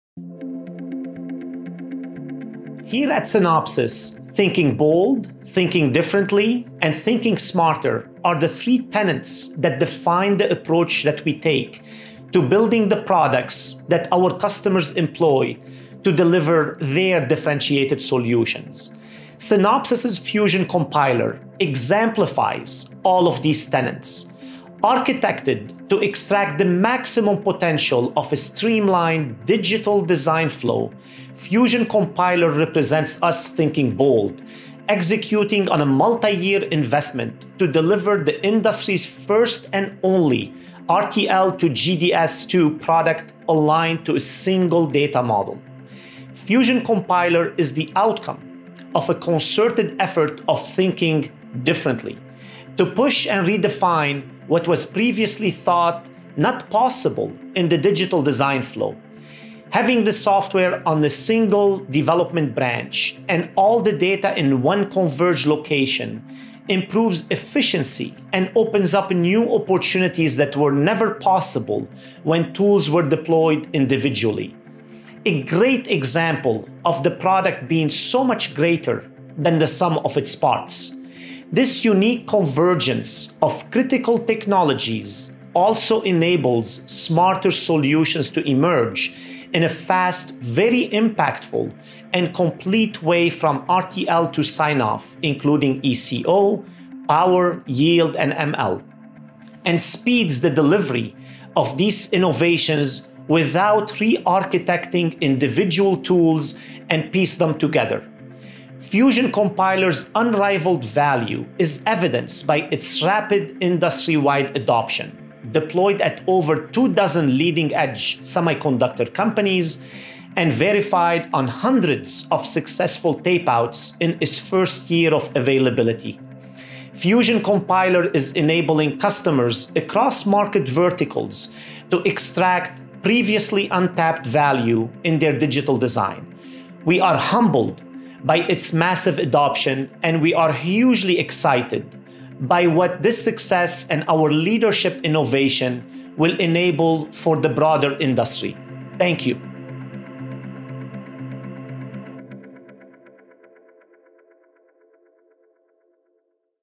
Bold, Different and Smarter: How Synopsys is Innovating to Keep You Ahead of the Curve
Sassine Ghazi, GM of Synopsys’ Design Group, describes the core tenets that shape the way we work at Synopsys: Thinking Bolder, Thinking Differently and Thinking Smarter. Discussing how a revolution in digital design was started by a big and audacious bet on creating a single data-model for all design, he goes on to share how Fusion Compiler, the industry’s only RTL-to-GDSII design product, was formed off this unique foundation and how this and other new, market-shaping innovations, are the result of this Synopsys-scale and Inimitably-Synopsys way of thinking.